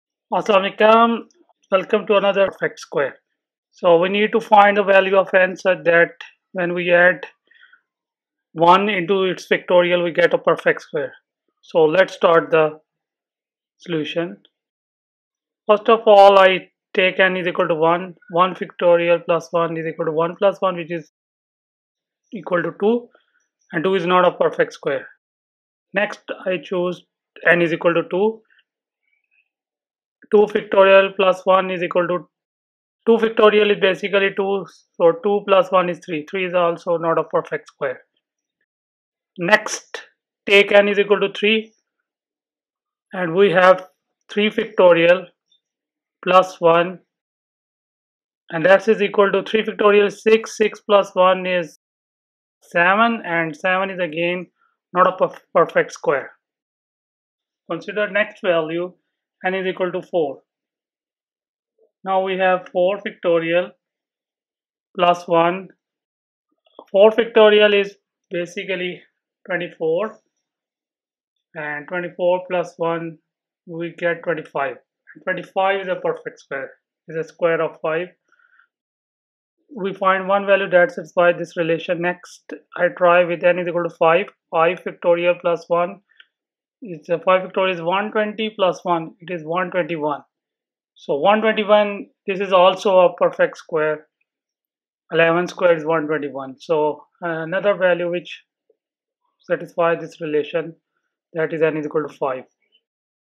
Unsolved Math Problem: When Is n!+1 a Perfect Square?
In this video, we explore the intriguing problem of identifying all positive integers n for which n!+1 results in a perfect square. Although this problem is unsolved in general, we analyse and verify the values of n up to 5 to see which ones satisfy this condition. Join us as we delve into factorials, and use perfect squares,  to investigate this fascinating mathematical challenge. Whether you're a math enthusiast or preparing for a math competition, this video will enhance your problem-solving skills and deepen your understanding of number theory.